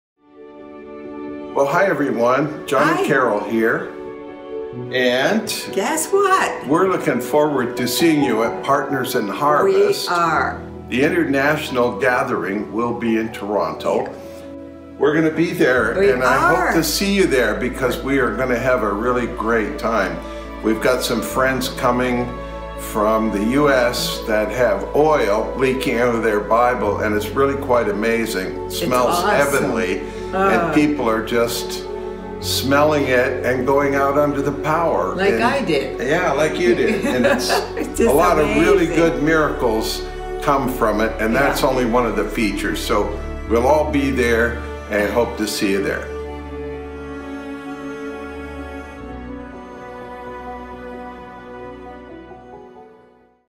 Greetings from John & Carol! PIH International Conf 2019
Please join us at our annual PIH International Conference 2019 in Toronto, Canada. Check for details and registration.